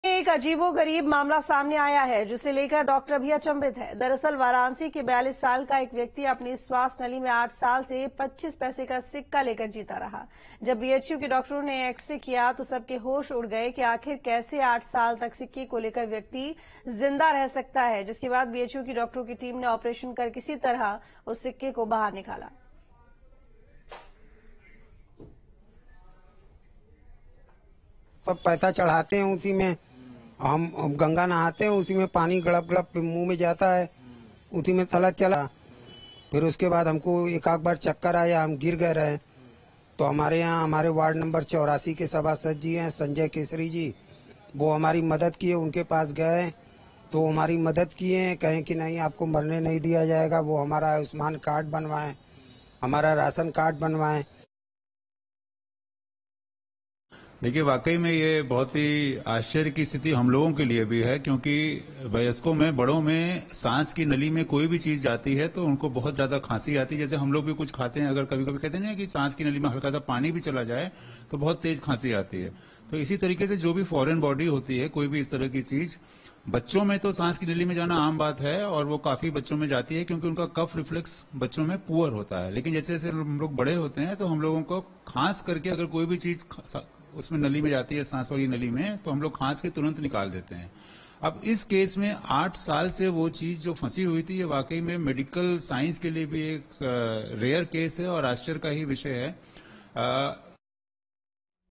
Varanasi से सामने आया हैरान करने वाला मामला, 8 साल से गले में फंसा था चवन्नी का सिक्का | BHU
News State Apps डाउनलोड करें:
Download Android and iOS Apps for more updates: